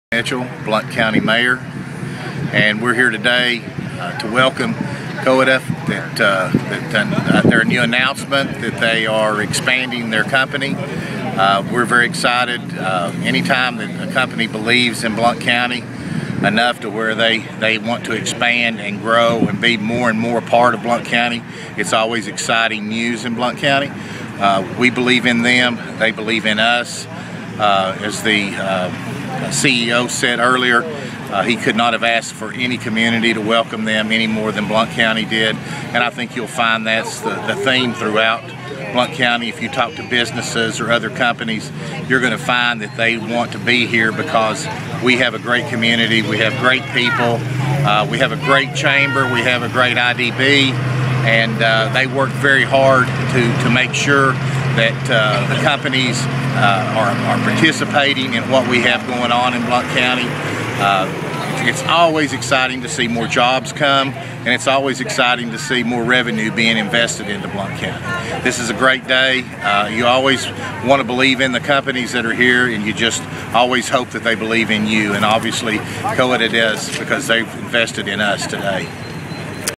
Mayor Ed Mitchell welcomes Koide Tennessee Inc
Blount County Mayor Ed Mitchell talks about Koide Tennessee expansion. (Video by Ed Marcum/News Sentinel)